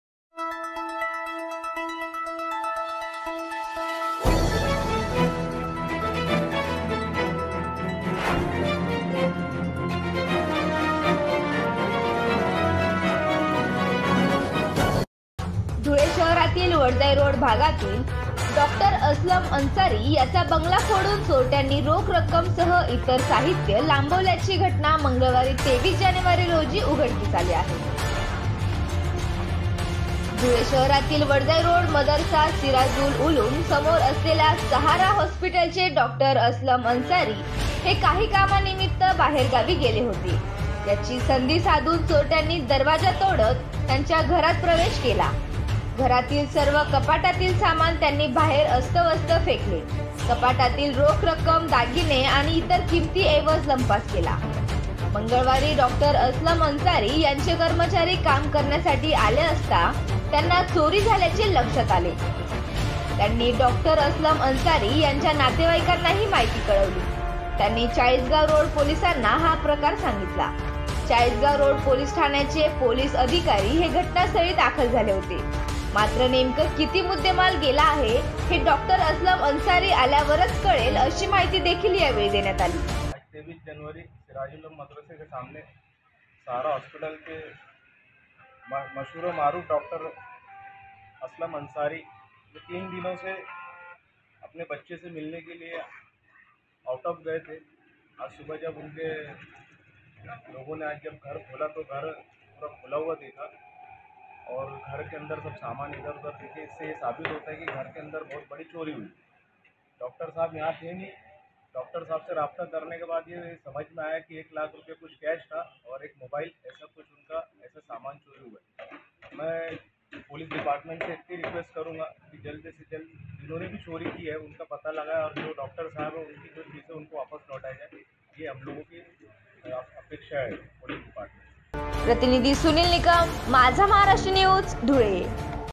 वडजाई रोड परिसरात डॉक्टरांच्या घरी चोरी लाखोंच्या ऐवज लंपास...
धुळे वडजाई रोड भागातील डॉक्टर असलम अन्सारी यांचा बंगला फोडून, चोरट्यांनी रोख रक्कम सह इतर साहित्य लांबवल्याची घटना मंगळवारी 23 जानेवारी रोजी उघडकीस आली आहे.